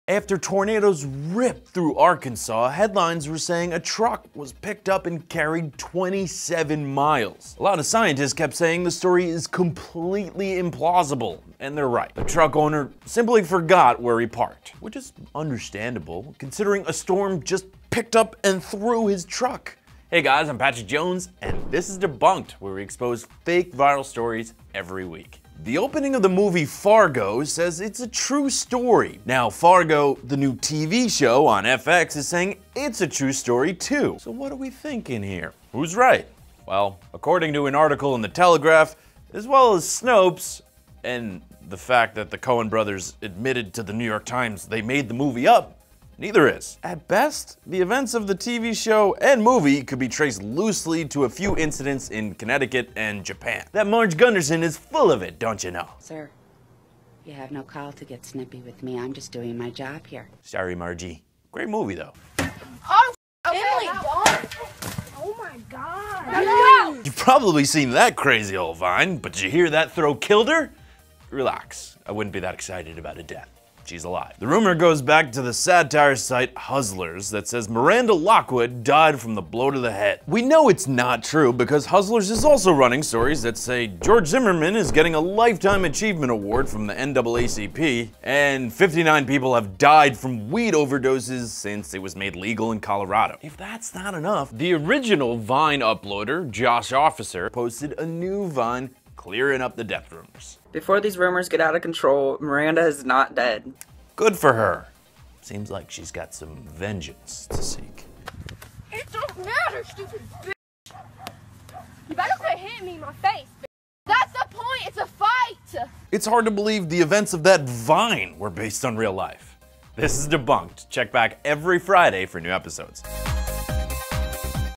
Debunk'd: Tornado Throws Truck 27 Miles, Vine Shovel Girl Dies, Fargo True Story
Welcome to Debunk'd: A new series recapping the top fake viral stories of the week, by Patrick Jones (@Patrick_E_Jones). In this episode we find out the tornado that recently ripped through Arkansas didn't toss a truck 27 miles, Fargo the movie and Fargo the FX TV series are not true stories, and the girl who was hit with a shovel is not dead.